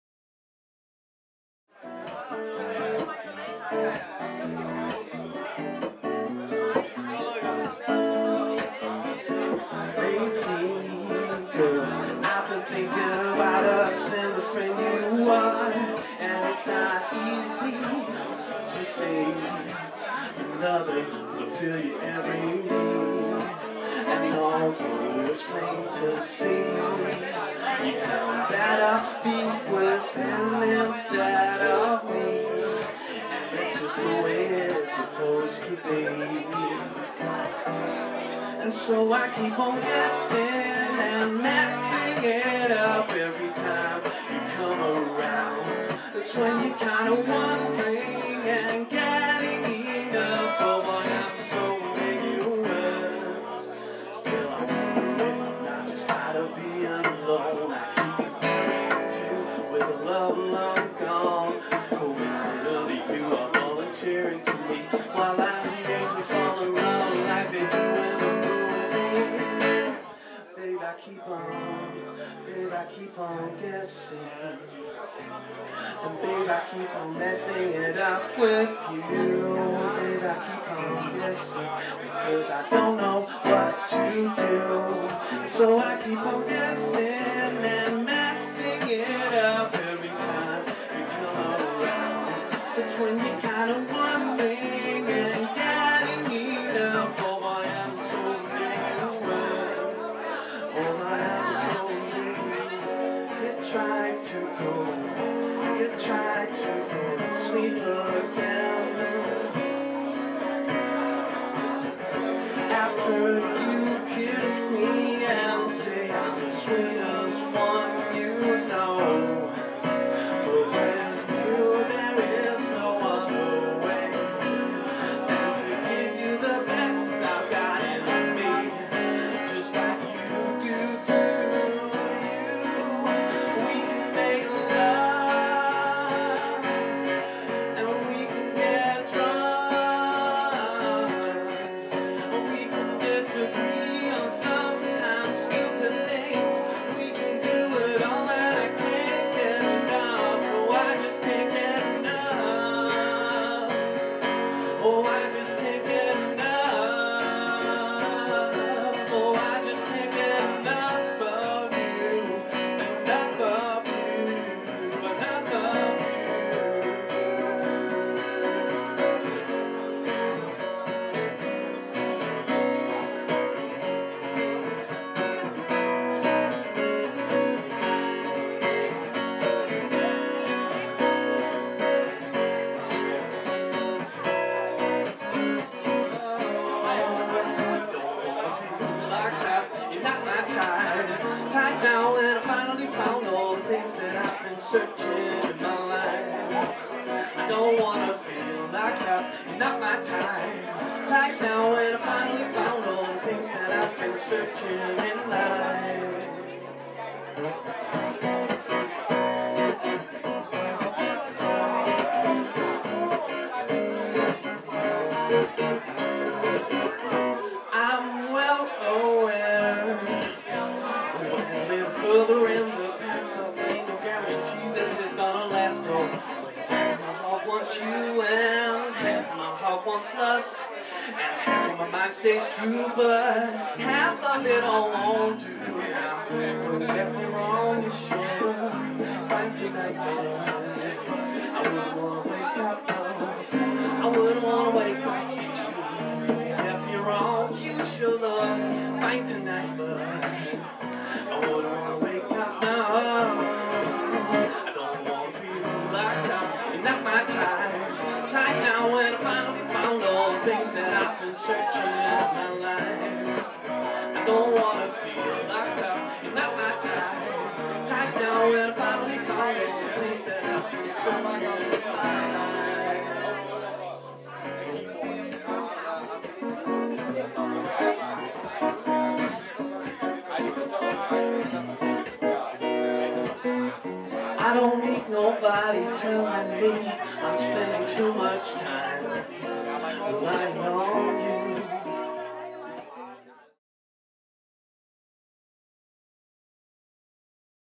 Tim Bakker - Fifteen Minutes LWD #2
Tim Bakker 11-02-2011, Fifteen Minutes LWD #2, Muziekcafe Scooter's, Leeuwarden.